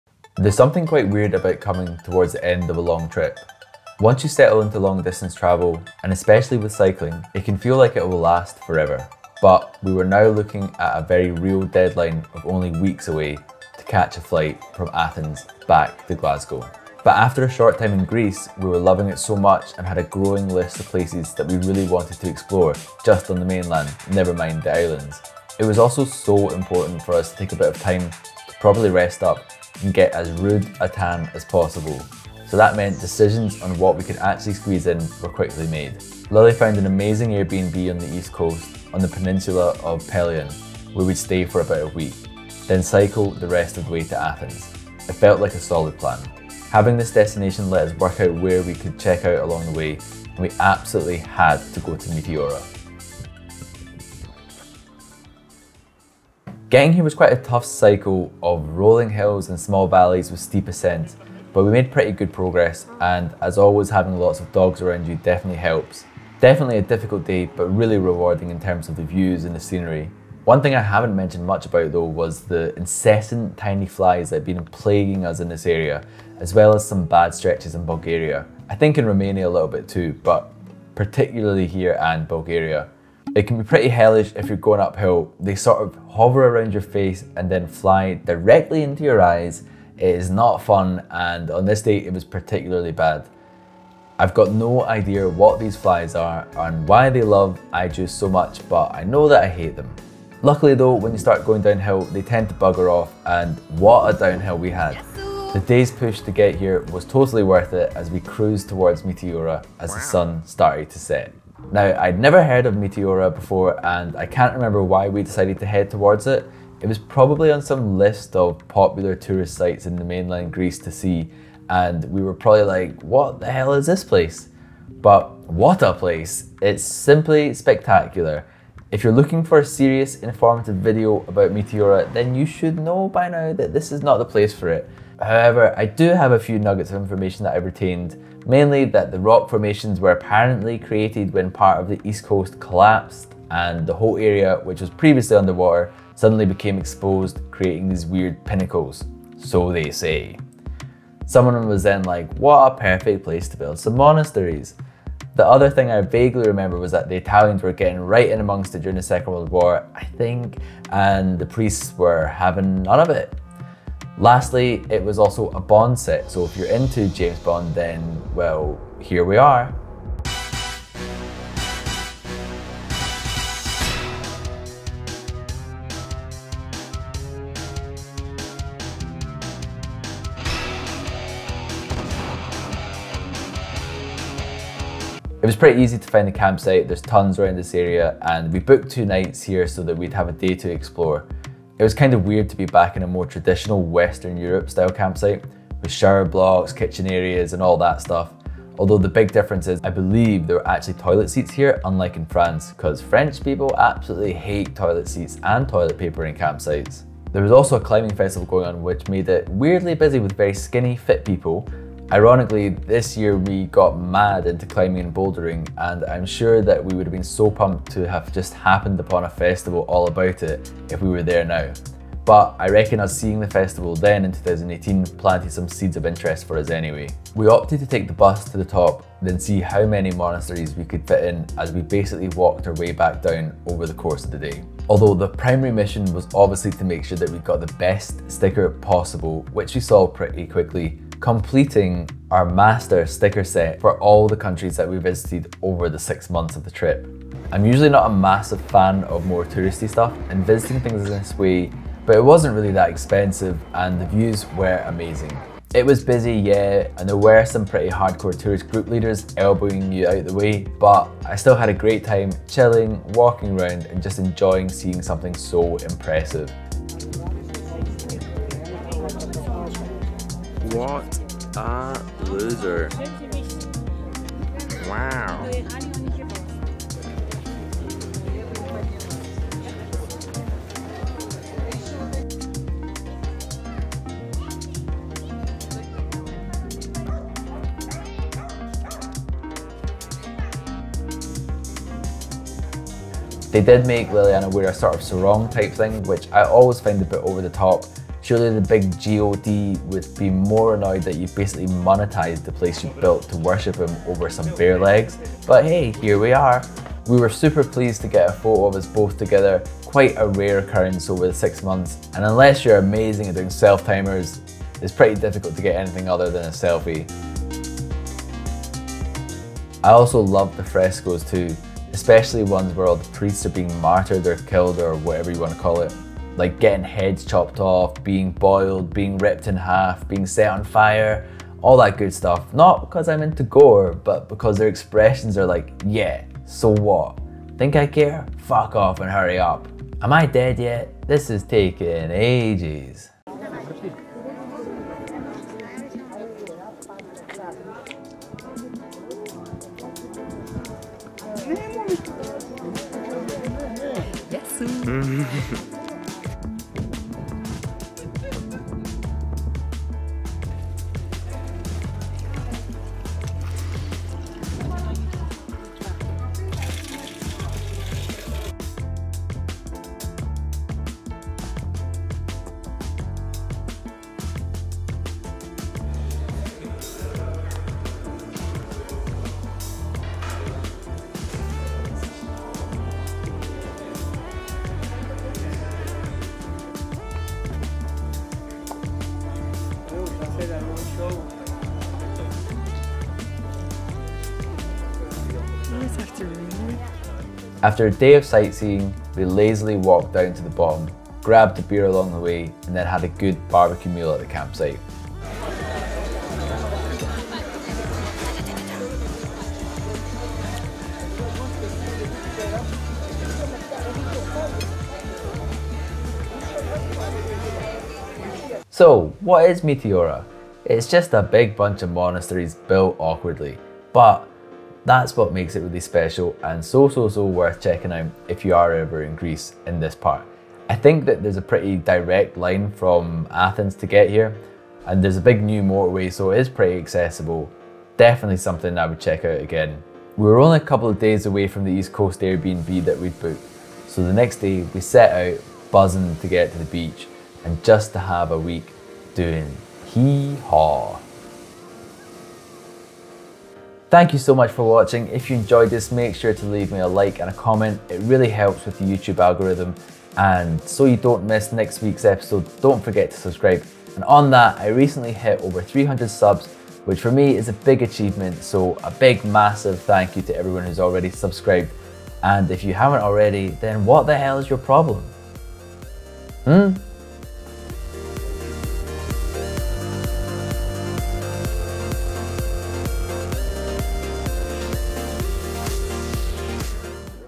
Meteora is a MUST see! | Bicycle Touring Greece
Meteora is an incredible sight to behold. Nestled into the side of mountainous areas on mainland Greece, the pinnacles and bizzare rock formations appear to come out of nowhere, and plopped on top some spectactular monasteries. It's no wonder they used them for a Bond Villain set!

We'd been cycling through Greece and were now pivoting towards the east coast where we'd booked an airbnb. That gave us plenty of time to go visit Meteora and see what all the fuss is about.
We opted to camp in the town at the bottom of the hill for two nights so we'd have a full day hassle free. Getting the bus to the top is pretty straightforward and we opted to go to the highest monastery first then walk our way back down. I'm not a massive fan of touristy destinations but honestly the views are stunning here, it's worth it just for the scenery. The monasteries are cool and well looked after but I don't really care that much about the history and very specific things, 2 years later I remember almost nothing about it. But I do know I had a great time and it was a highlight of our bicycle touring adventure!

We used this tent:
MSR Mutha Hubba NX